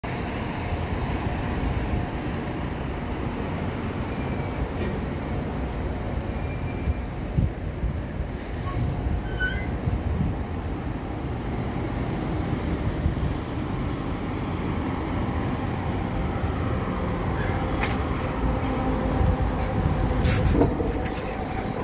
Transperth 2747 arrives at Stop 10155
Transperth 2747 (Volvo B7RLE/Volgren Optimus) operated by Path Transit (Redcliffe), seen pulling into Stop 10155 of Albany Highway after John Street, Bentley.

Filmed With: Samsung Galaxy Note 10+ (SM-N975F)